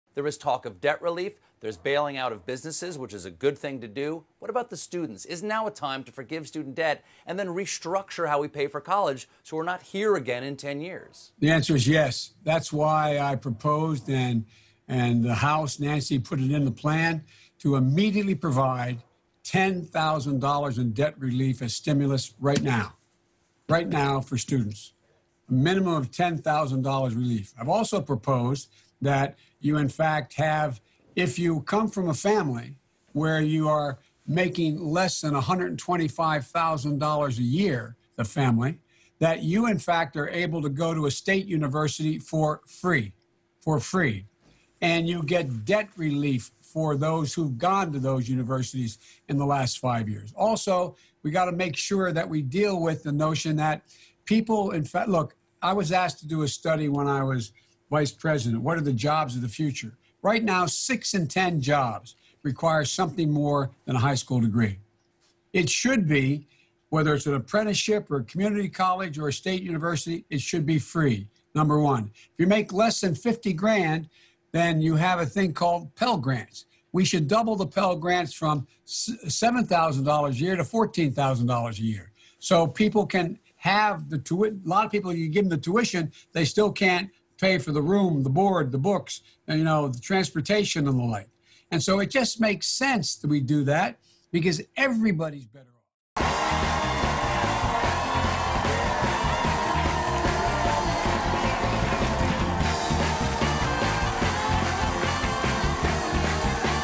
Joe Biden: I Propose We Immediately Provide $10k In Student Loan Debt Relief
Democratic presidential candidate Joe Biden details his proposal to immediately provide student loan debt relief as part of his larger plan for economic stimulus tied to the Covid-19 crisis.